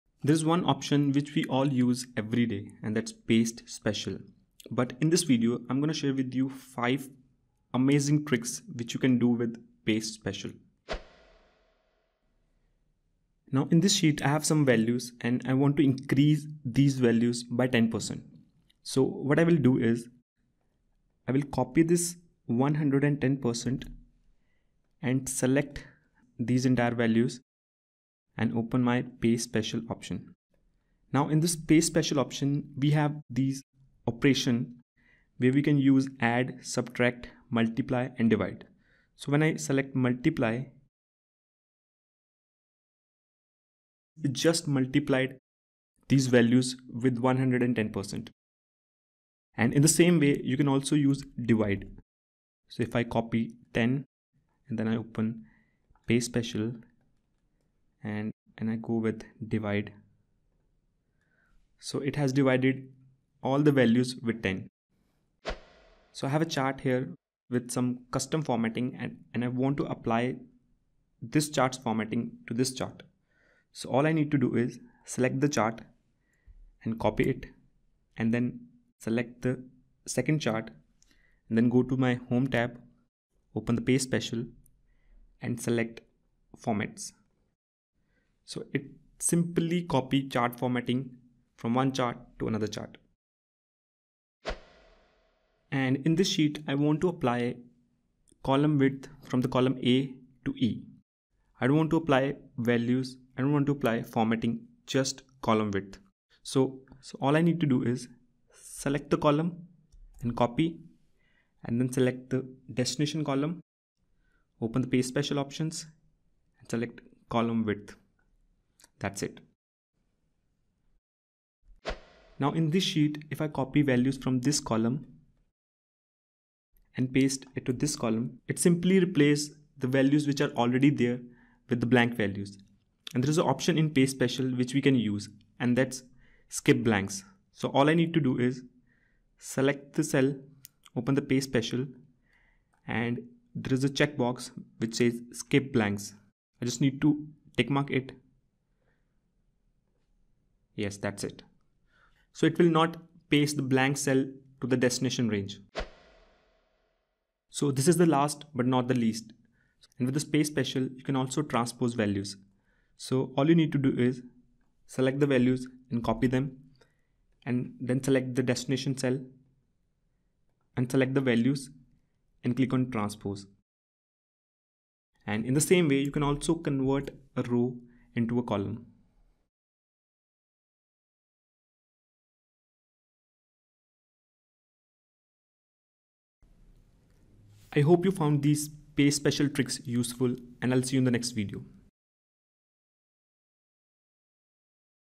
5 Excel PASTE Special Tricks You Need to Know 💡
There's one option which we all use every day and that's PASTE Special. But in this video, I'm gonna share with you 5 amazing tricks which you can do with paste special.